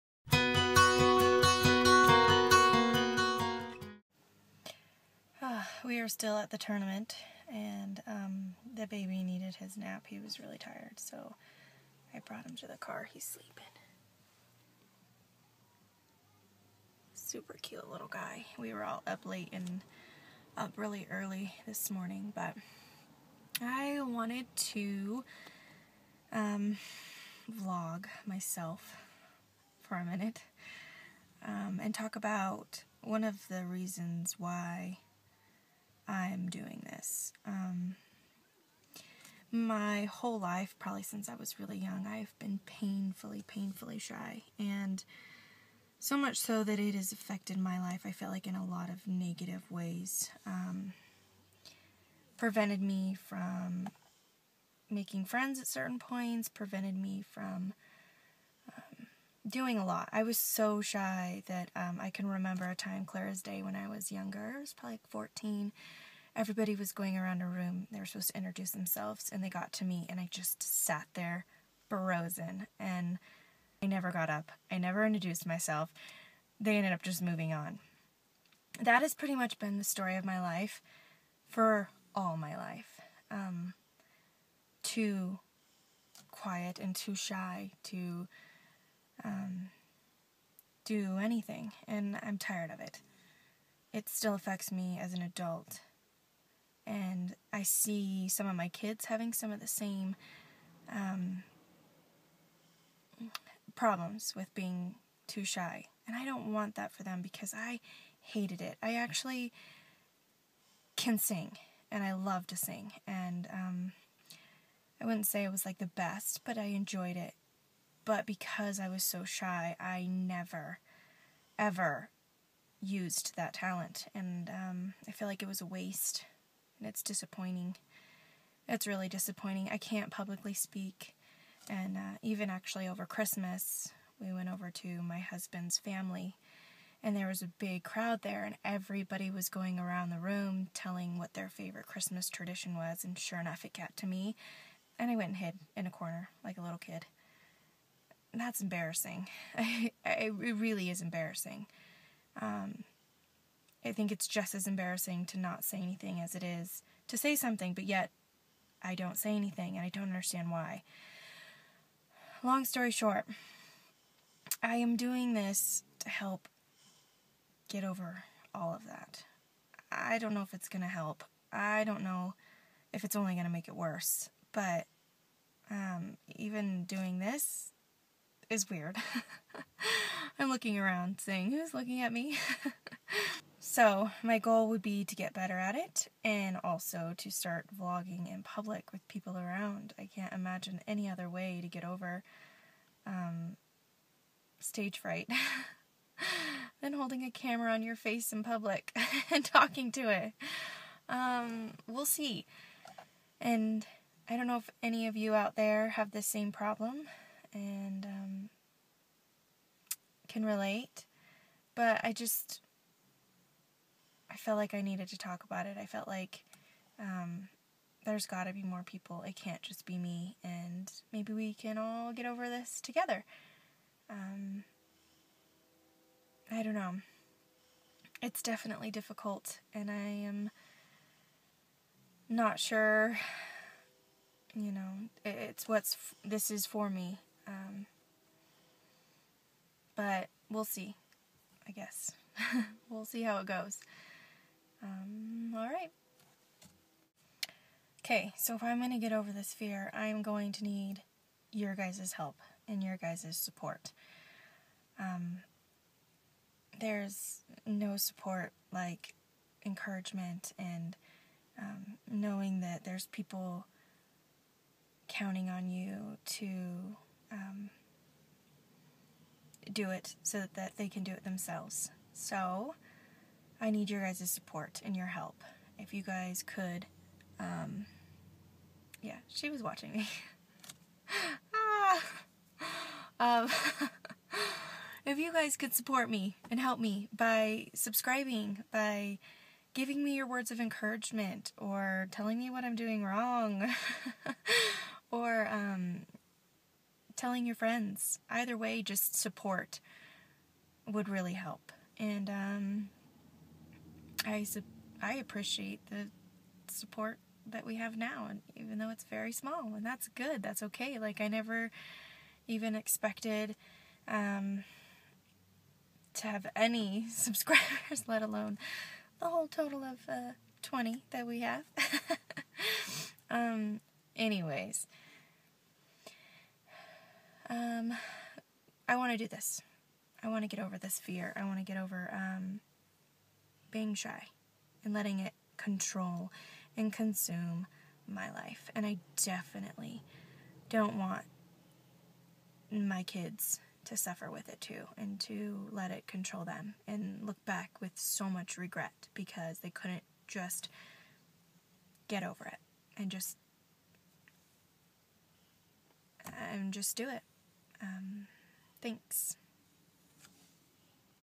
Are you shy?
I am stepping out of my comfort zone and talking to you guys about something I have struggled with my whole life. This was not easy for me to do and hope you guys can support me through it. Thanks